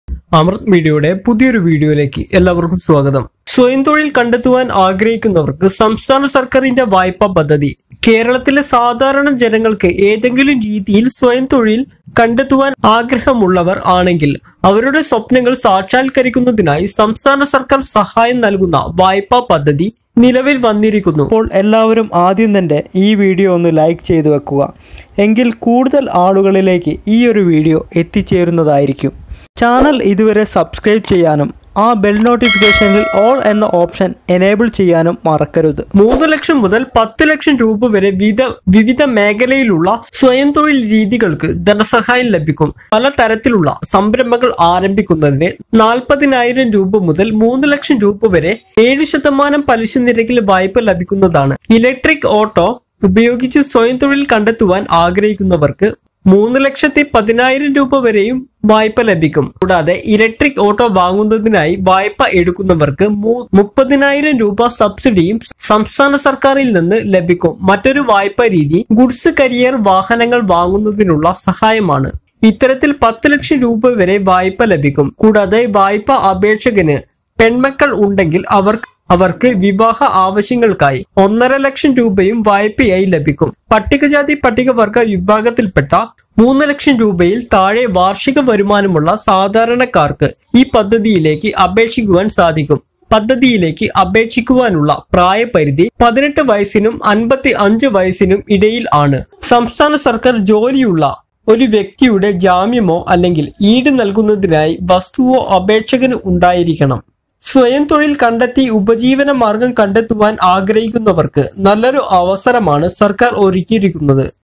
സ്വയം തൊഴിൽ കണ്ടെത്താൻ ആഗ്രഹിക്കുന്നവർക്ക് സംസ്ഥാന സർക്കാരിന്റെ വായ്പാ പദ്ധതി | Amrith media
Those who want to find self-employment should know more about the loan scheme of the state government

For the common people of Kerala, if they want to find a job in any way, the State Government has introduced a Loan Scheme to help them realize their dream.

Funding for self-employment schemes in various fields ranging from Rs. 3 lakhs to Rs. 10 lakhs.  Loans ranging from Rs 40,000 to Rs 3 lakh are available at 7% interest to start various ventures.  Those who want to find self-employment with Electric Auto can get a loan of up to Rs 31,0000.

In addition, those who take loans to buy electric autos will get a subsidy of Rs 30,000 from the state government.  Another type of loan is financing the purchase of a goods carrier vehicle.

Loans up to Rs 10 lakh can be obtained in this manner.  In addition, if the loan applicant has daughters, they can get a loan of Rs 1.5 lakh for marriage purposes.  Ordinary persons belonging to Scheduled Castes and Scheduled Tribes with an annual income of less than Rs. 3 lakhs can apply for this scheme.

The age limit to apply for the scheme is between 18 and 55 years.  The project is currently underway in Ernakulam district.  The applicant must have the security or collateral of a person holding a state government job.

The state government has provided a good opportunity for those who want to find self-employment and earn a living.